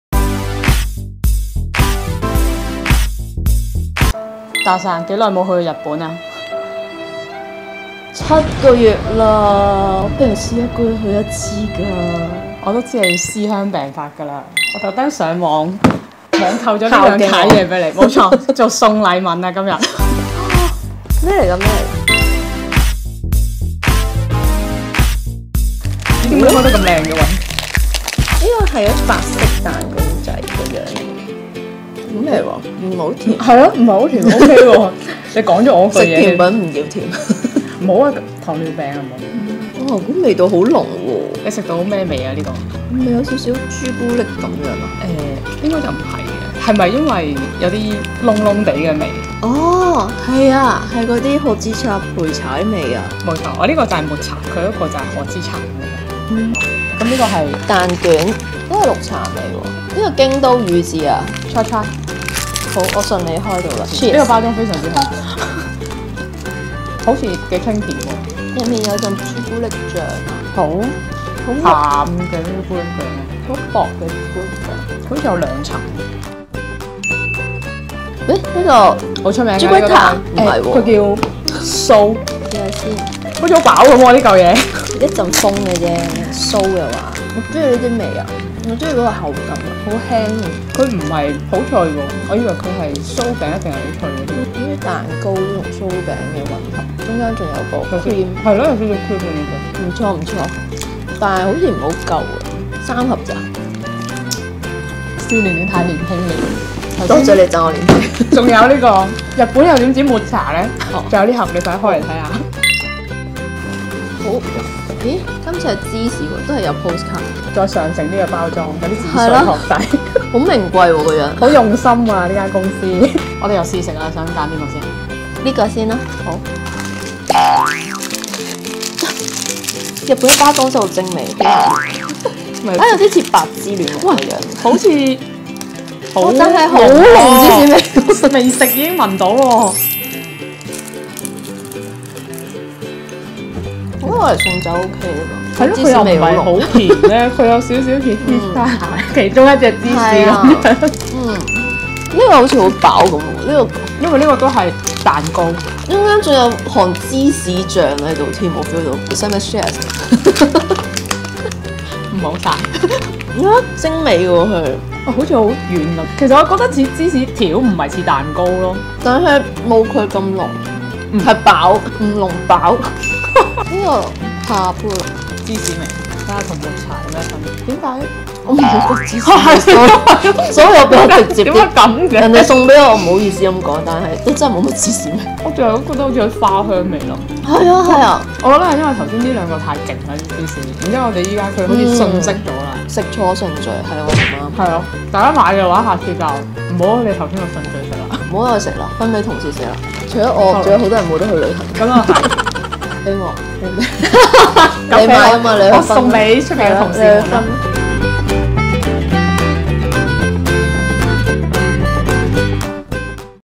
【網購開箱】在家耍廢網購 試食6款日本人氣抹茶、芝士零食
好掛住日本！要一解思鄉病，不如網購一堆日本零食返屋企，邊睇劇邊歎，食咗當去咗。

全方位緊貼《U Magazine》社交平台